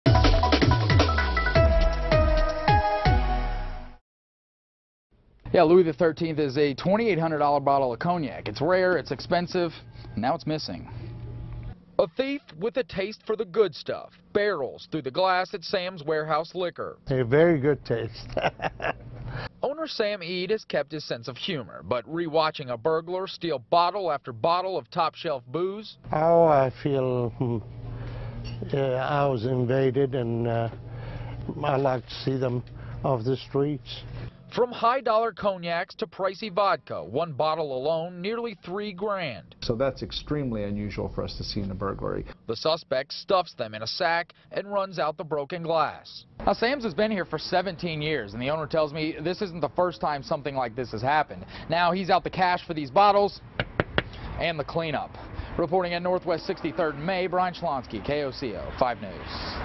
$2,800 bottle of booze stolen from OKC liquor store
A man reportedly stole a bottle of liquor valued at more than $2,800 Saturday from a store in northwest Oklahoma City.